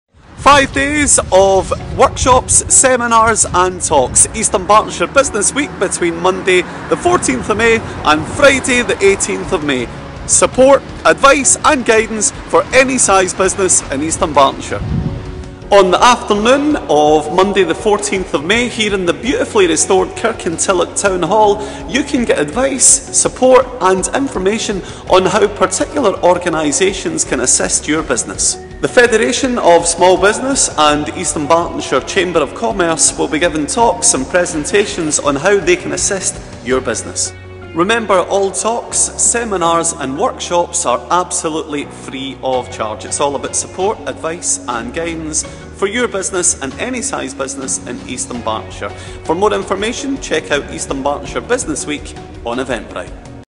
East Dunbartonshire Business Week - Kirkintilloch
A look ahead to East Dunbartonshire Business Week at Kirkintilloch Town Hall - Mon 14 May 2018. for details.